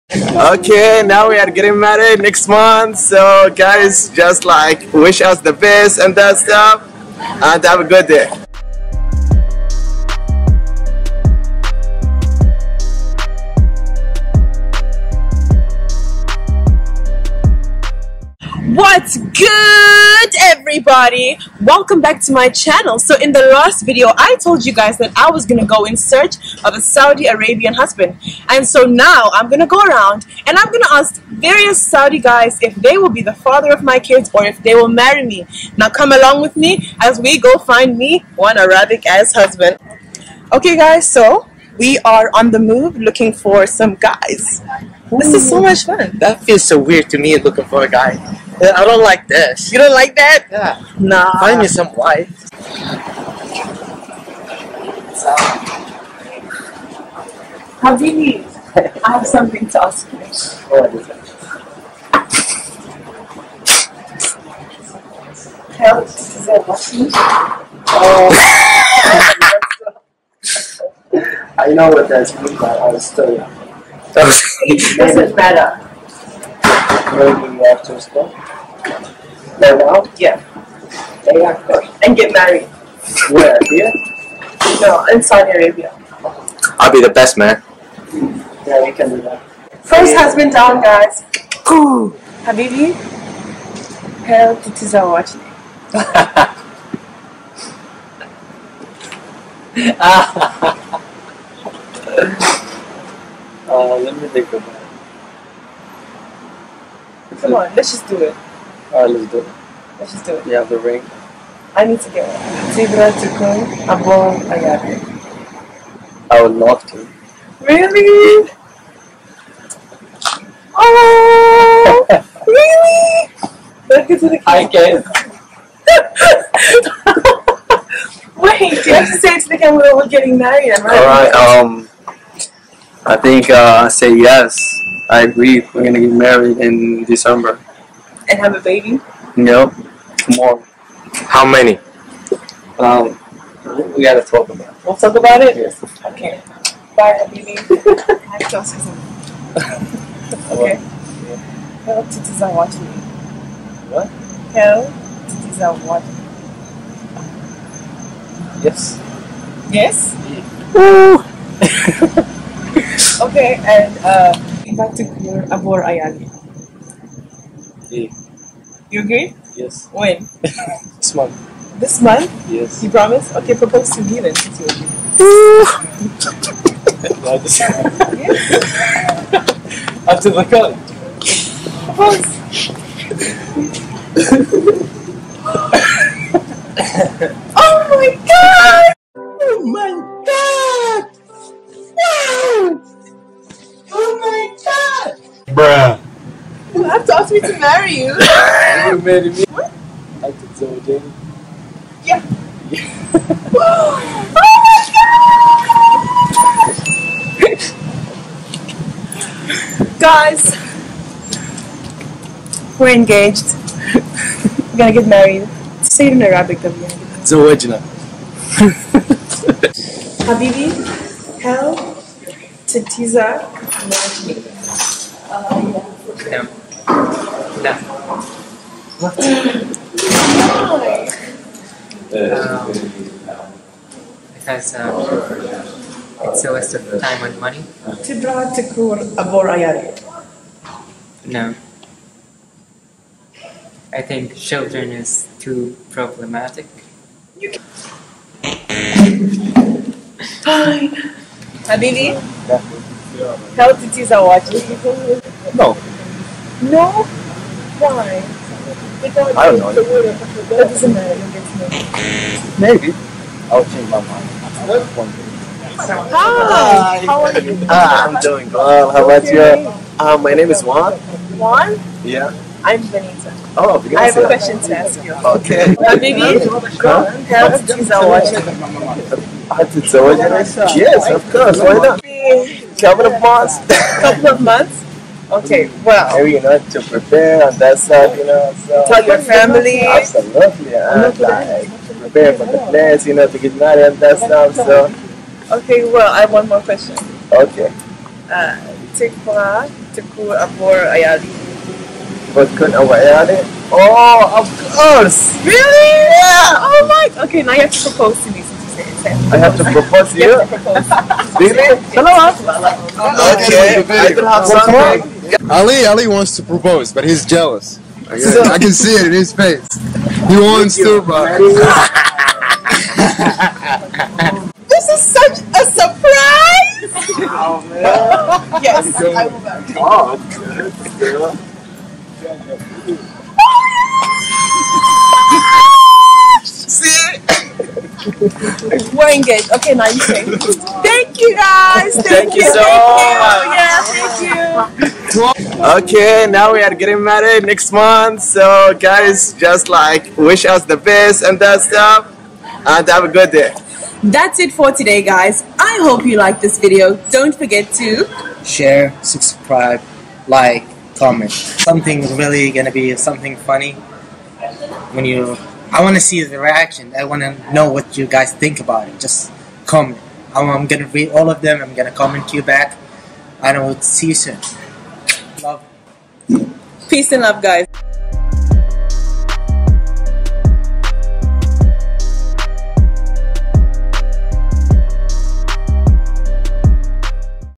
PROPOSING TO SAUDI ARABIAN GUYS IN ARABIC || SOUTH AFRICAN YOUTUBER
I'M ENGAGED!!